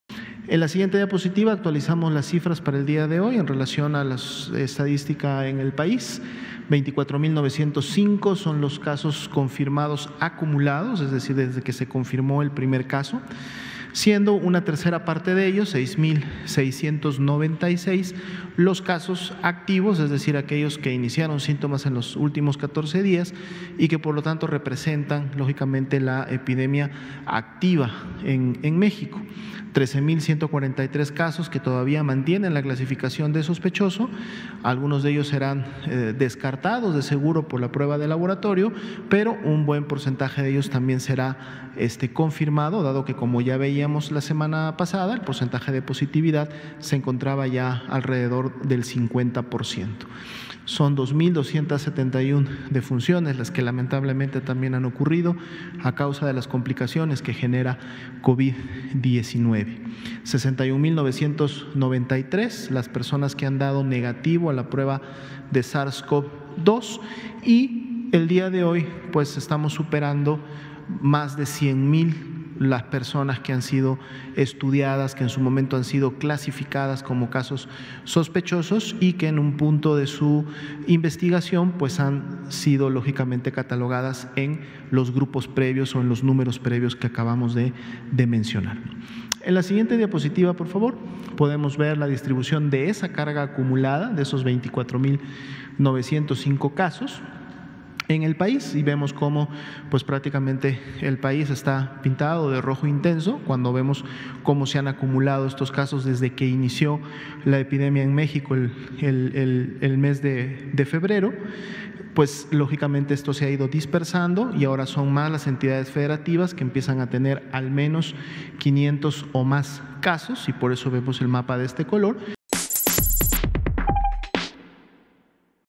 Registra el país 24,905 casos de coronavirus y 2,271 defunciones
El director General de Epidemiología, José Luis Alomía, informó que en México hay 6,696 casos activos, 13,143 casos sospechosos, 61,993 pruebas negativas y 100,041 personas estudiadas.